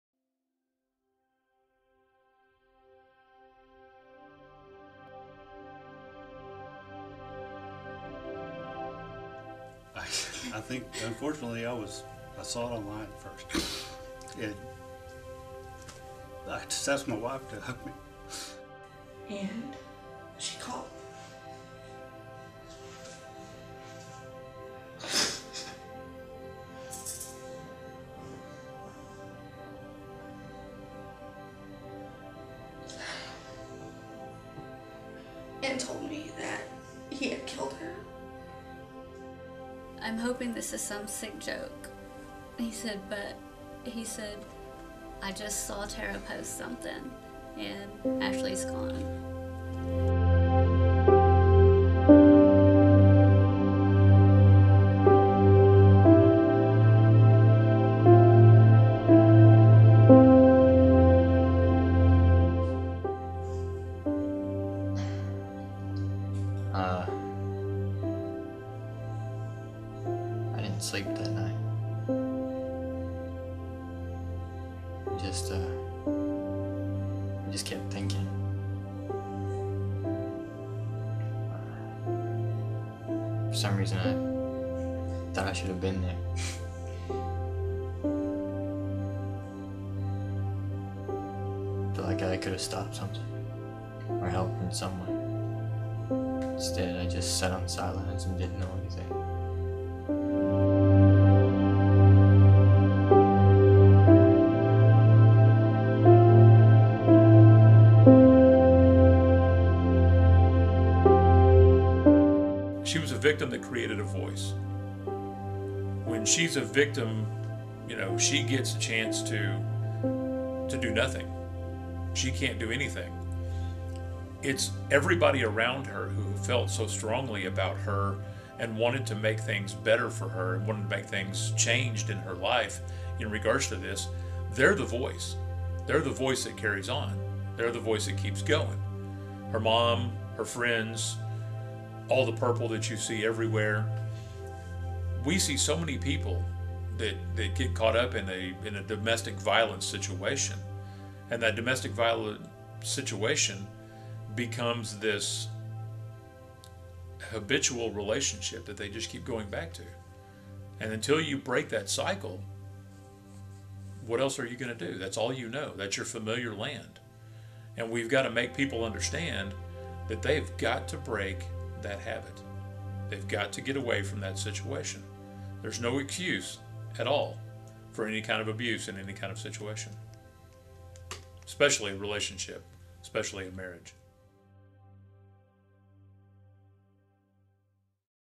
Ashleigh's Patience Project: The Documentary
The story of Ashleigh Lindsey and Domestic Violence Awareness